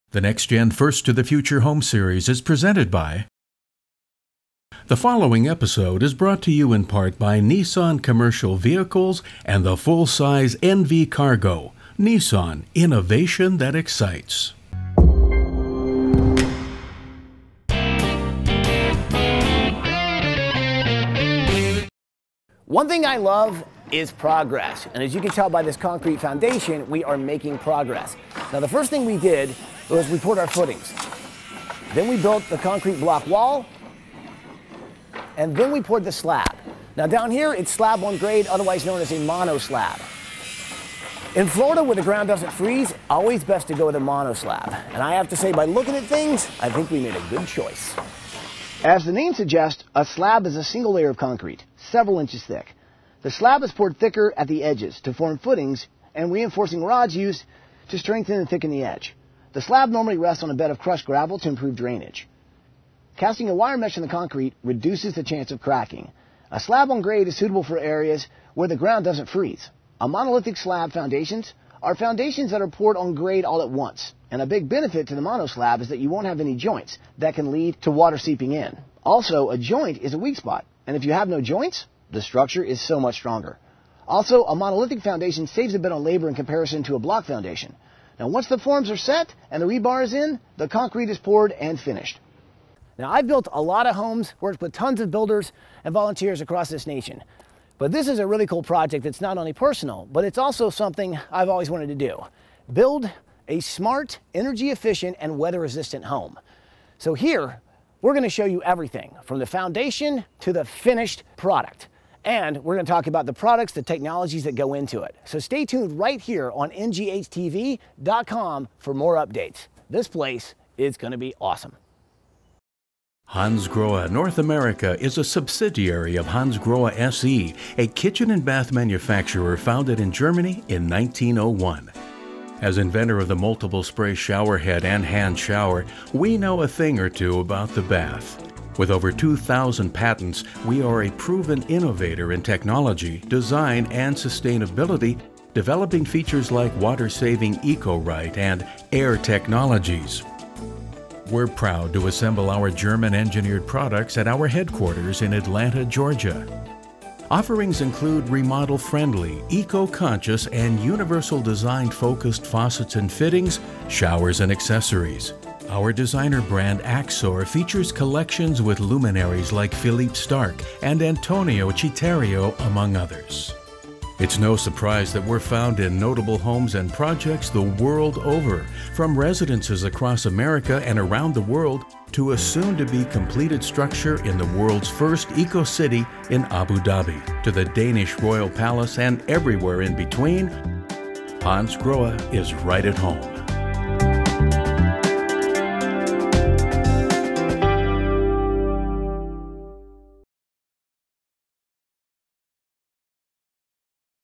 EP 7 - The Monolithic Slab Foundation.
The FIRST TO THE FUTURE® home keeps growing.  On the lower foundation we poured a traditional slab foundation.  But on the upper level we poured a Mono Slab.